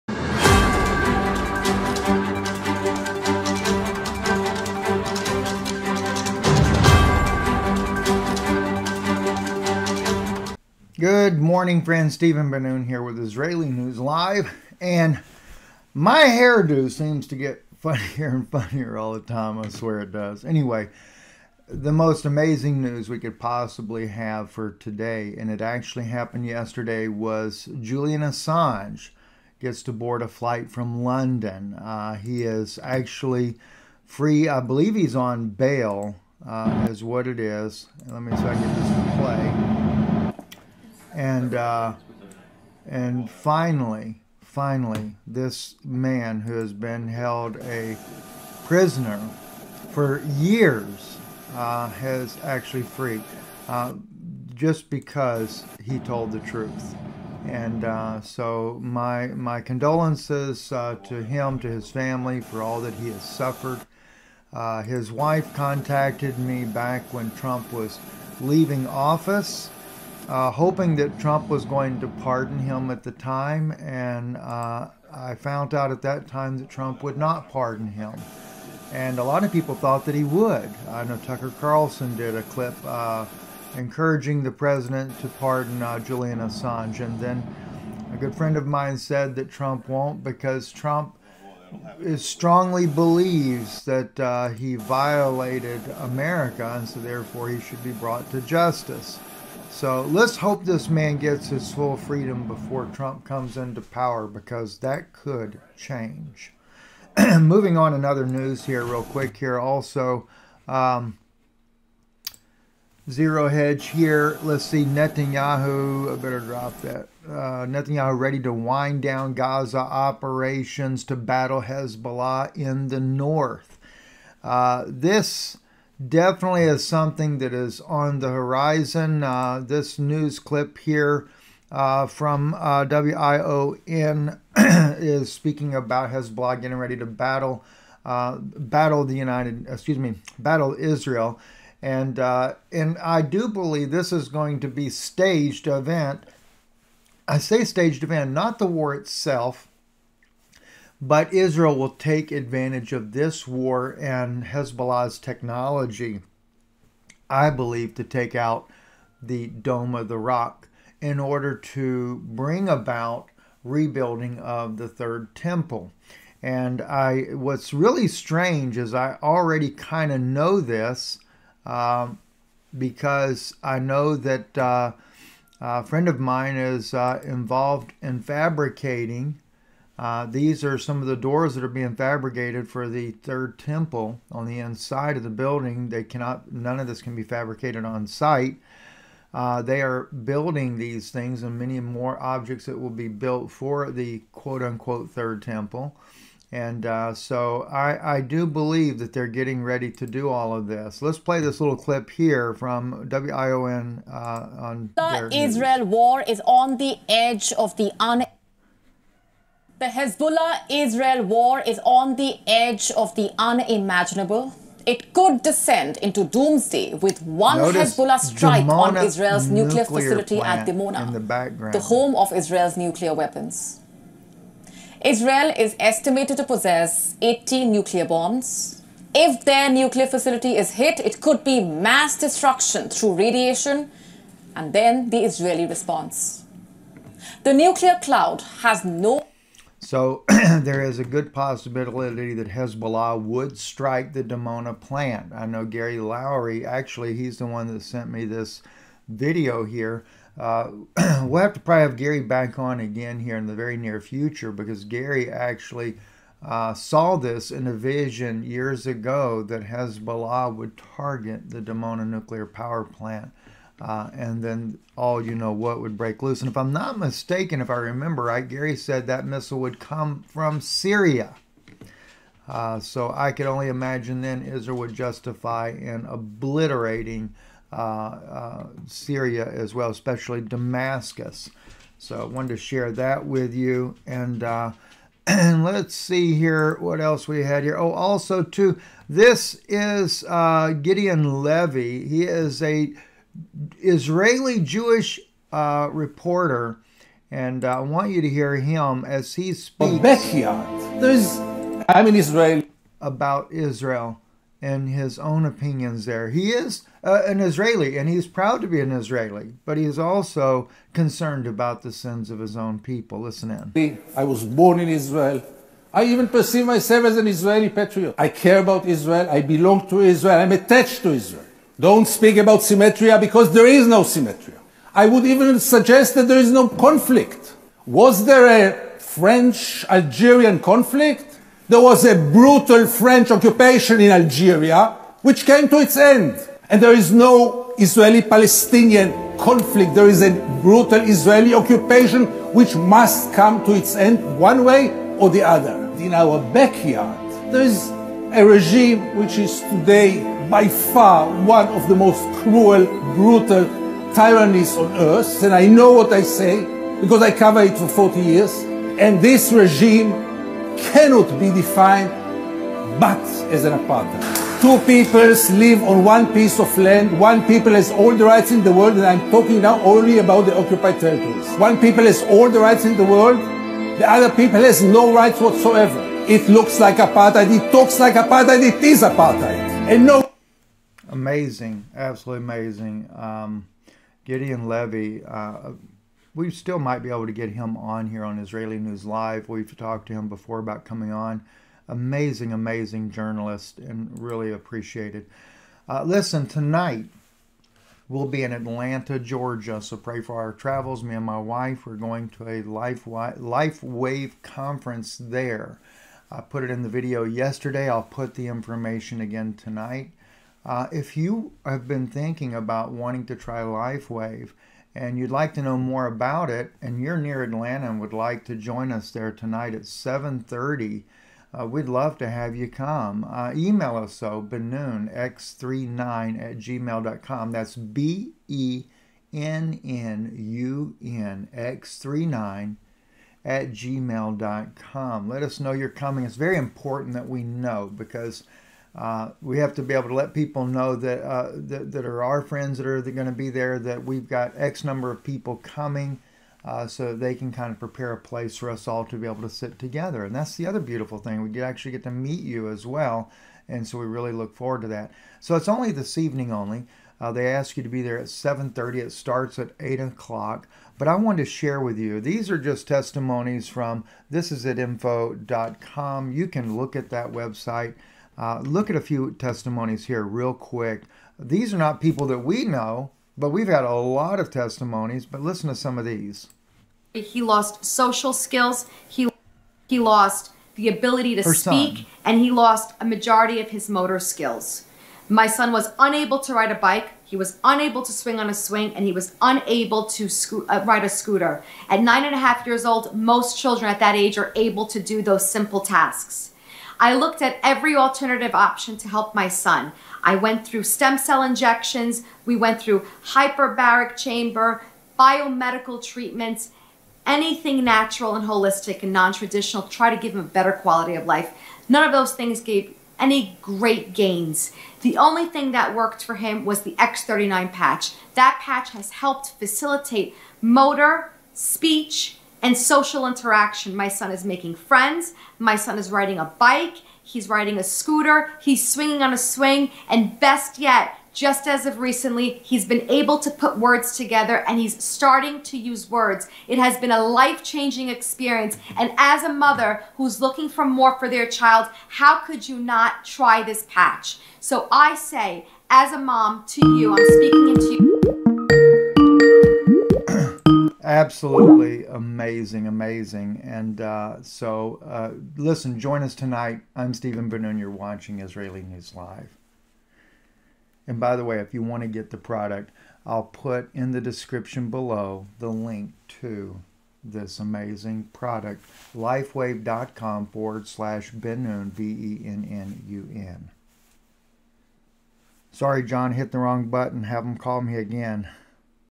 Julian Assange is Free
We often take Freedom of Speech for granted and for Julian Assange he and his family have paid dearly for Freedom of Speech.

Double Tree by Hilton Hotel
7:30 PM Tuesday night June 25th
Atlanta-Northlake,
Lavista Rd Atlanta Georgia